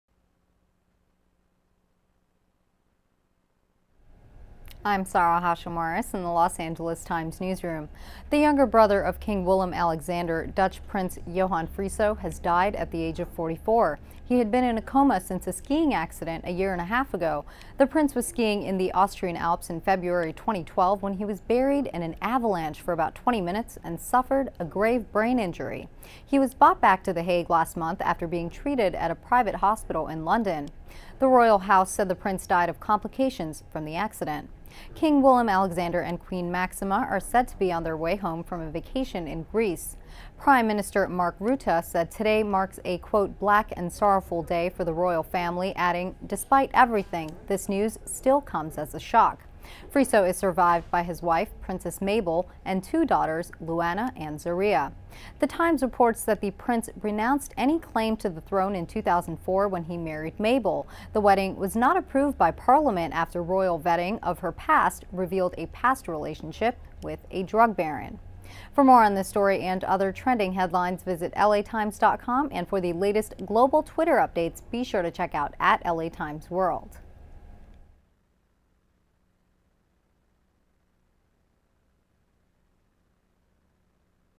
Headline: Dutch Prince Johan Friso dies after 18 months in coma
Reporter Sarah Hashim-Waris has details on the death of 44-year-old Dutch Prince Johan Friso, who was in a coma since a skiing accident a year and a half ago; the royal house said he died from complications from the accident.